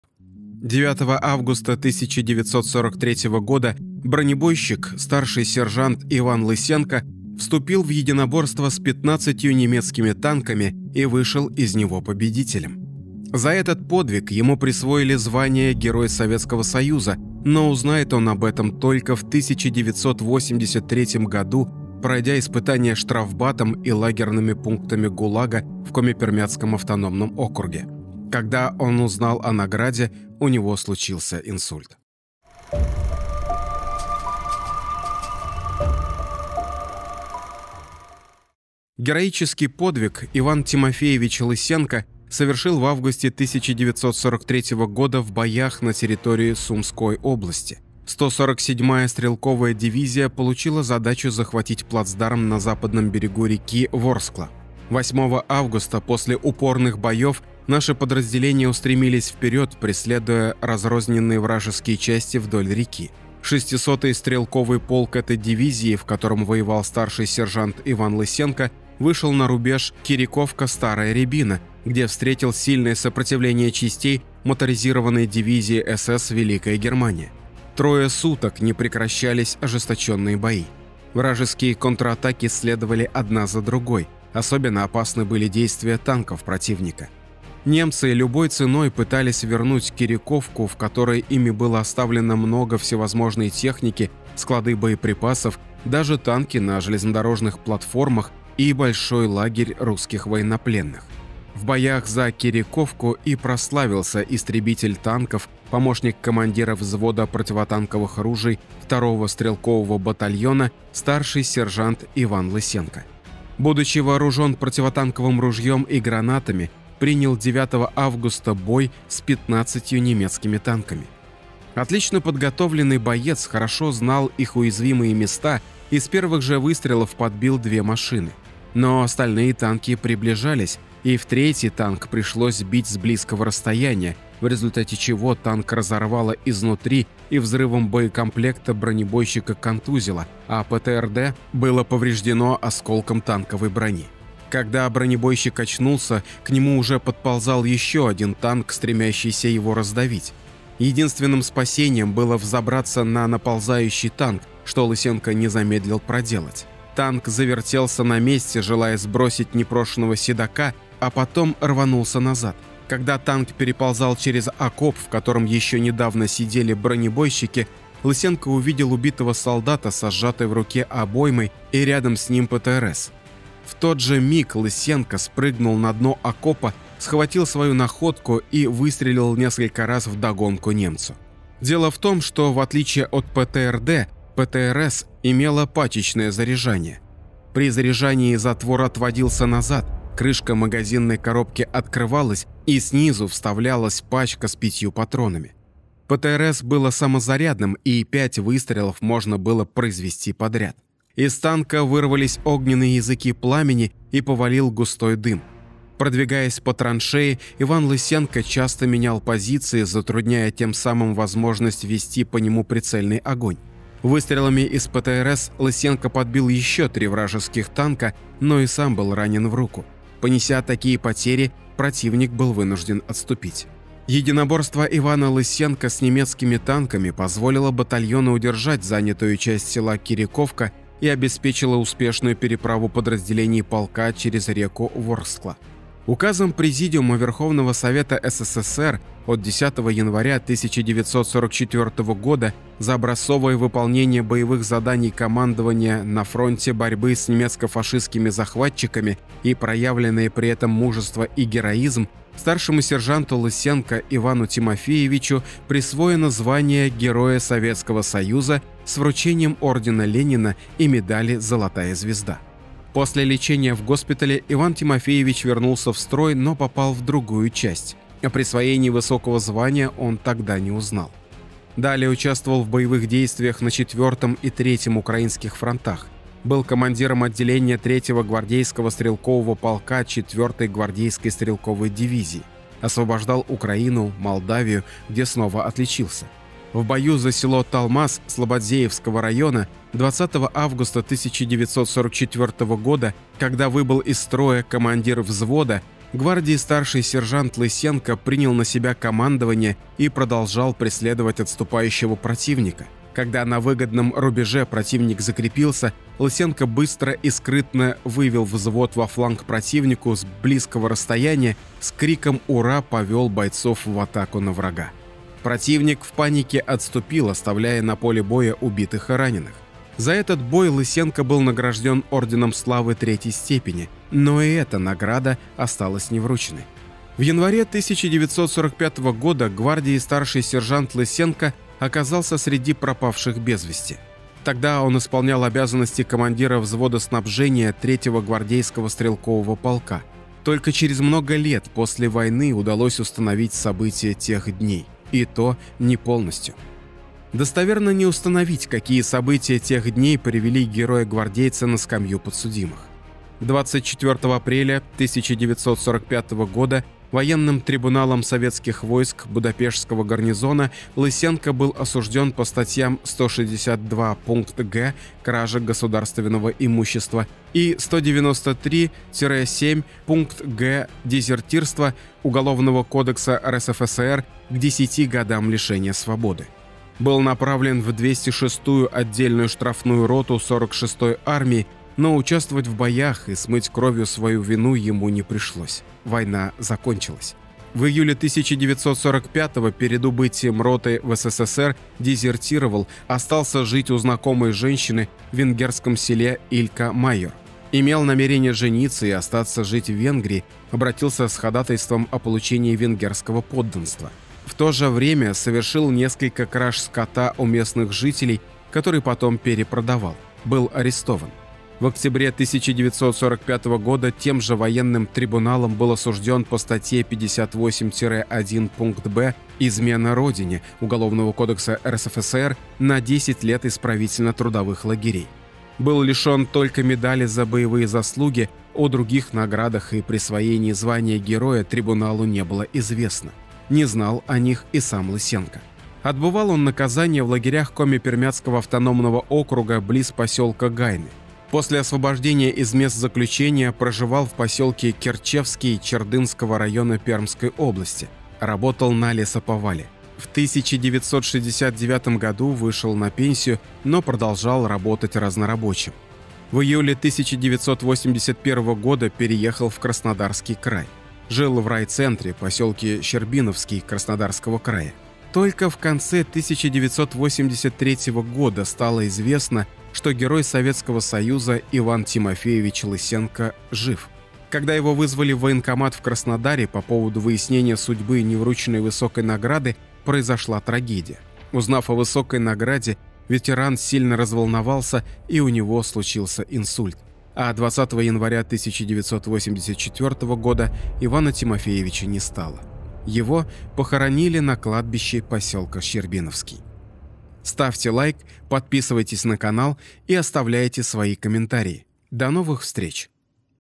В ОДИНОЧКУ против 15 танков! Лысенко Иван Тимофеевич
9 августа 1943 года бронебойщик старший сержант Иван Лысенко вступил в единоборство с пятнадцатью немецкими танками и вышел из него победителем. За этот подвиг ему присвоили звание Герой Советского Союза, но узнает он об этом только в 1983 г., пройдя испытание штрафбатом и лагерными пунктами ГУЛАГа в Коми-Пермяцком автономном округе. Когда он узнал о награде, у него случился инсульт…

Герои Советского Союза: Краткий биографический словарь / Пред. ред. Коллегии И.Н. Шкадов — М.: Воениздат, 1987. — Т. 1